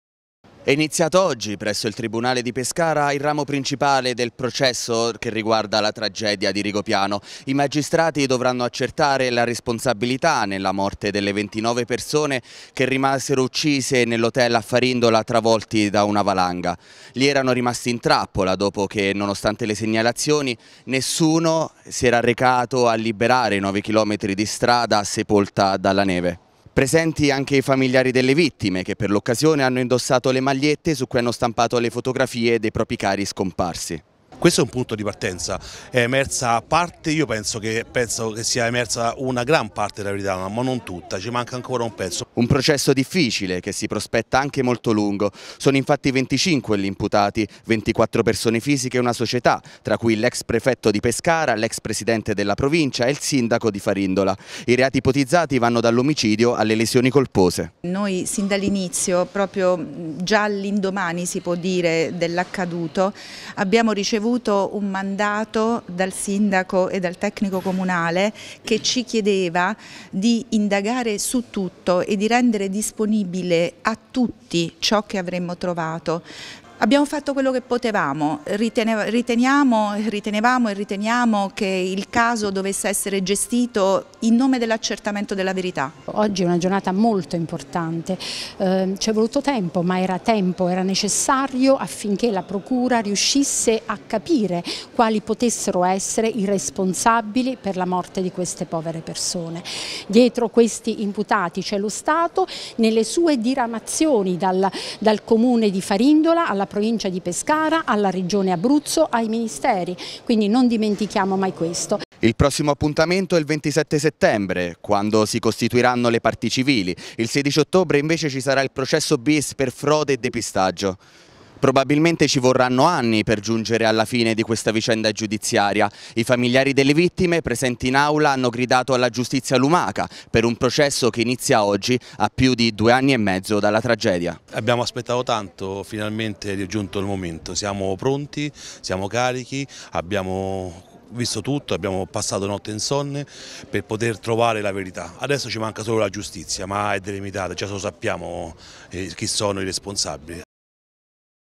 Rigopiano. Si torna in aula il 27 settembre
Al via il processo riguardo la tragedia di Rigopiano. 25 gli imputati totali. Presenti in aula anche i familiari delle vittime che hanno mostrato le immagini dei propri cari scomparsi sotto la valanga a inizio 2017